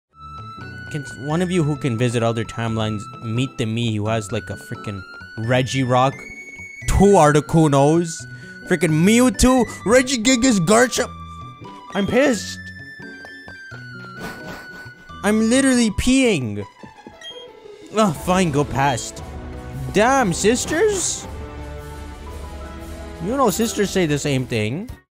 "Can someone meet the me who..."
TTar consoles all the lost Legendary encounters he could've had while stuck with a Tentacruel and a Toddler.